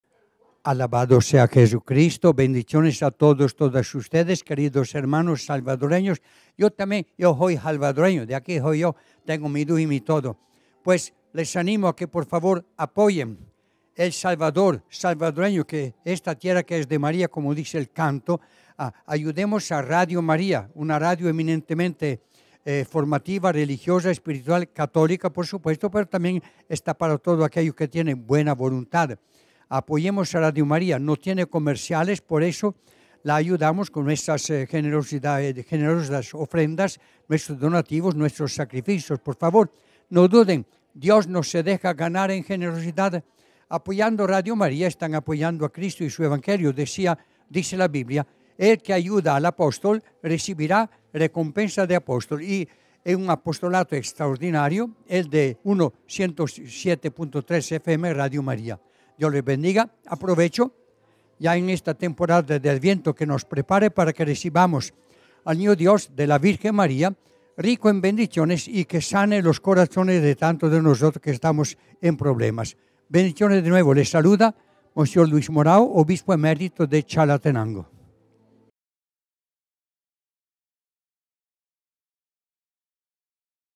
Monseñor Luis Morao en cena navideña 2017 de Radio María El Salvador
Santísimo Films - Una llave de la Evangelización -
Santísimo Films ©